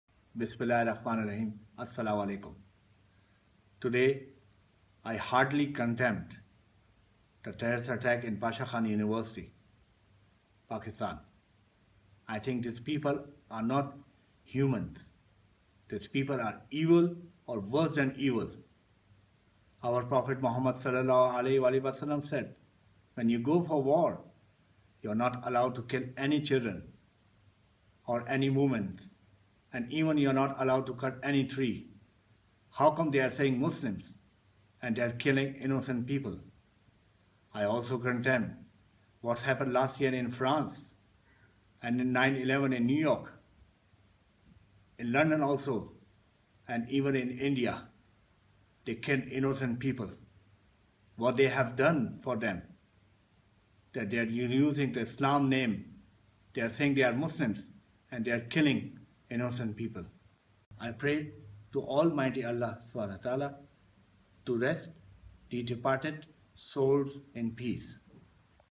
Suhail Muhammad Al Zarooni Sharing His Views and Condemning Recent Terror Attacks in Pakistan!
May Allah (swt) Grant Courage and Solace to the Parents and Mothers to Bear this Irreparable loss with Equanimity. May Allah (swt) Protect Everyone (Ameen) we all should Stand United and fight against Terrorism Charsadda. Prayed to Almighty Allah (swt) to rest the departed souls in Eternal Peace. We share their Pain and offer our Deepest Condolences. The Entire Muslim World is in Grief over Pakistan Tragedy Happened Today & at this Moment of Grief. I Vehemently Condemned the Terrorist Attack in Bacha Khan University & that the World at large is in Search of Peace.